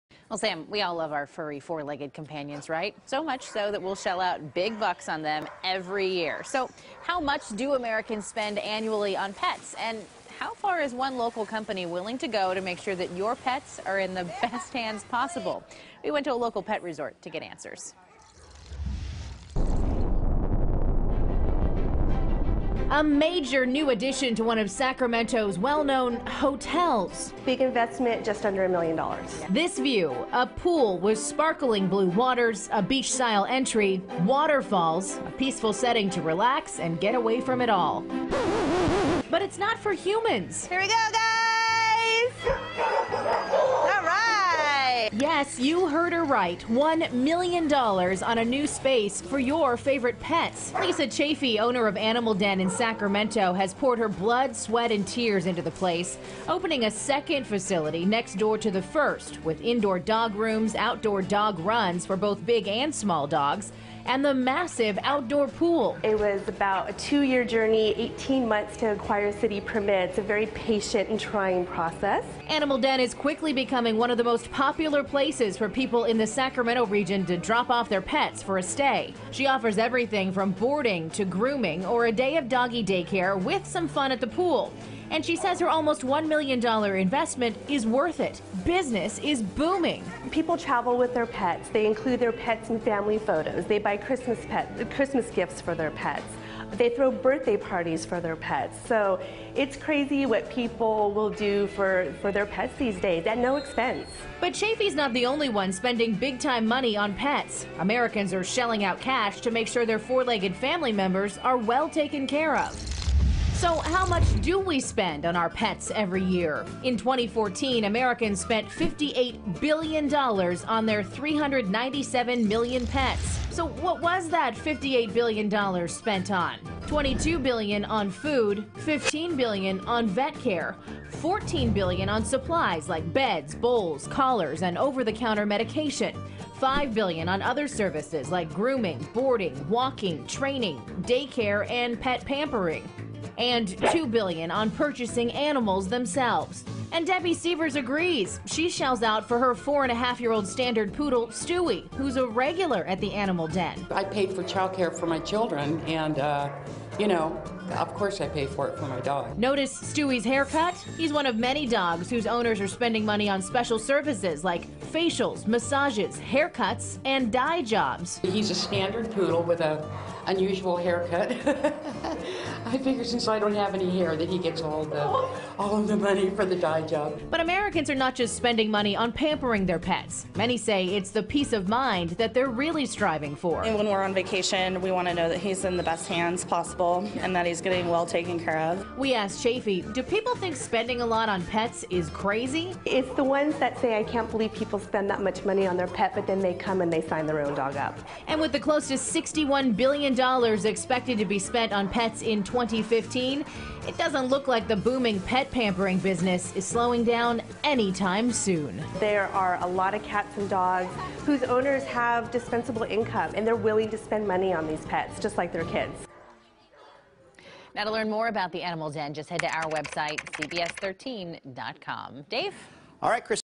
Sacramento Pet Hotel Takes $1 Million Plunge As People Spend More On Animals
Lisa Chafee, owner of Animal Den in Sacramento has poured her blood, sweat and tears into the place, opening a second facility next door.